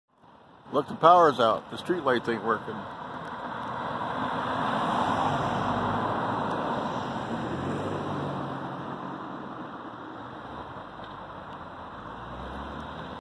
Gene Bowdish, Power outage Whole west side of Olympia, WA Is out of power Refrigerators Freezers Etc
Gene Bowdish
Power outage
Whole west side of Olympia, WA
Is out of power
Refrigerators
Freezers
Home medical life saving equipment, etc
Do you have a UPS ?
Uninterruptible Power Supply (UPS)